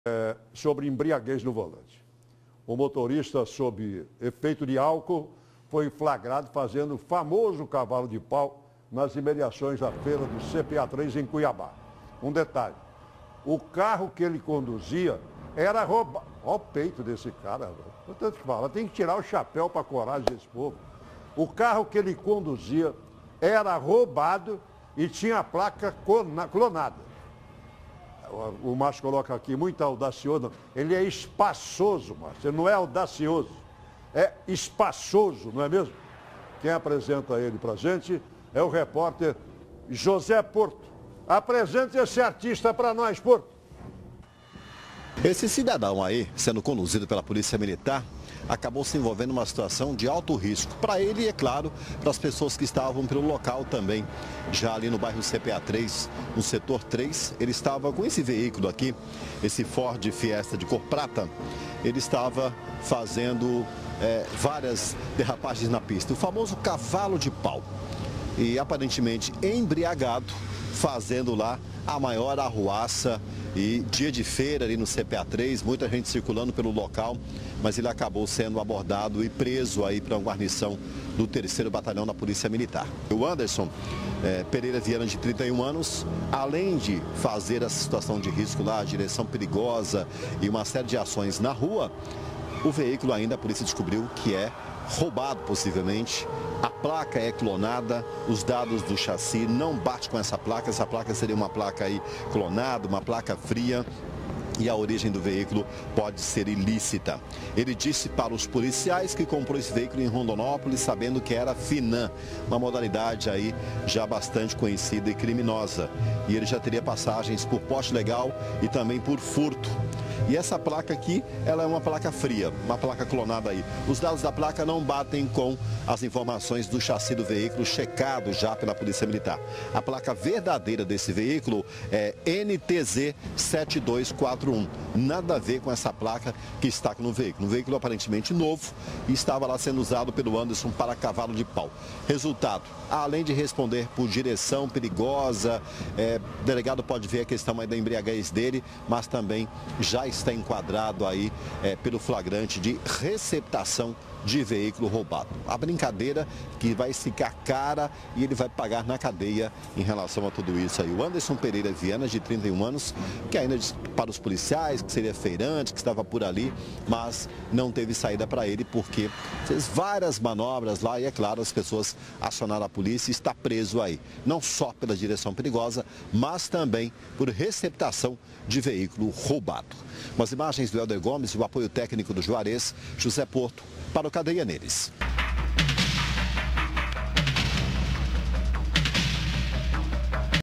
Motorista é flagrado sobre efeito de álcool e fazendo cavalinho de pau
O fato ocorreu no CPA 3, o carro que ele conduzia era roubado e tinha placa clonada.